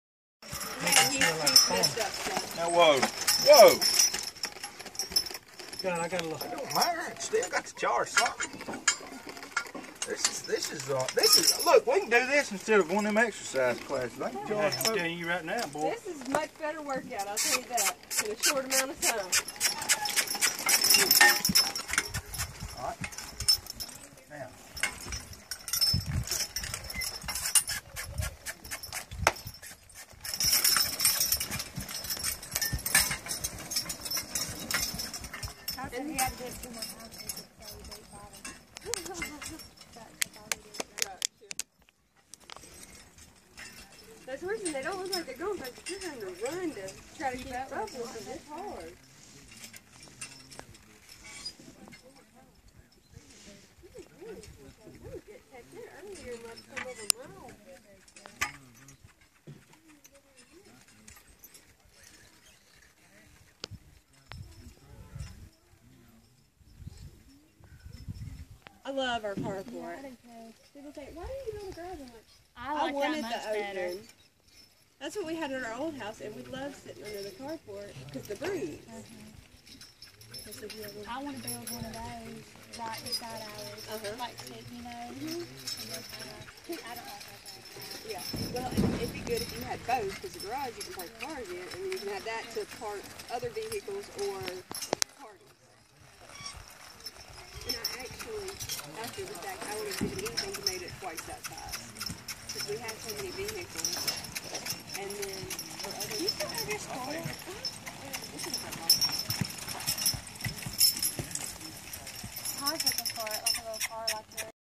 Breaking Ground the ol fashioned way
Scott's massive Belgiums doin work, with a little help from Todd. Hard work boys. Hard work.